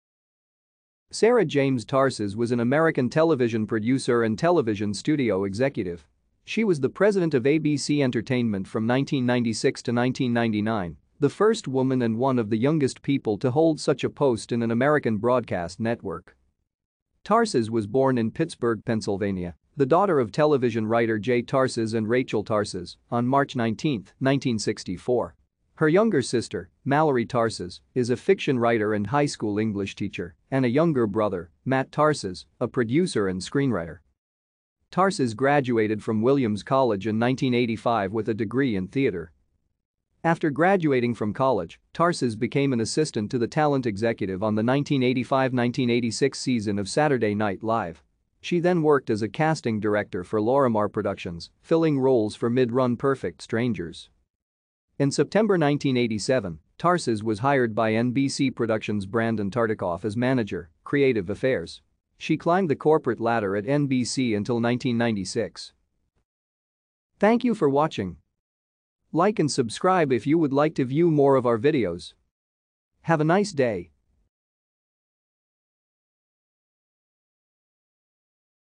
Jamie Tarses
Sara James Tarses  was an American  producer and  studio executive. She was the president of ABC Entertainment from 1996 to 1999, the first woman and one of the youngest people to hold such a post in an American broadcast network.